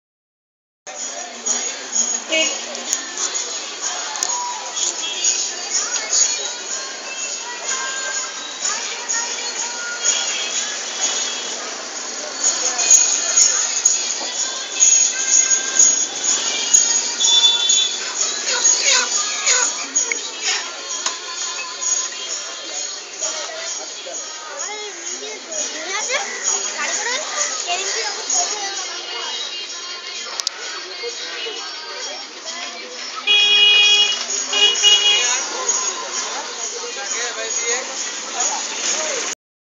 VEGETABLES & FRUITS MARKETS.
VEGETABLES  &  FRUITS MARKETS.IT IS A FAMOUS MARKET OF EAST KOLKATA  .EVERYBODY COMES  THIS MARKET  REGULARLY TO BUY FRESH VEGETABLES ,FRUITS, MILK, MEAT,FISH ETC.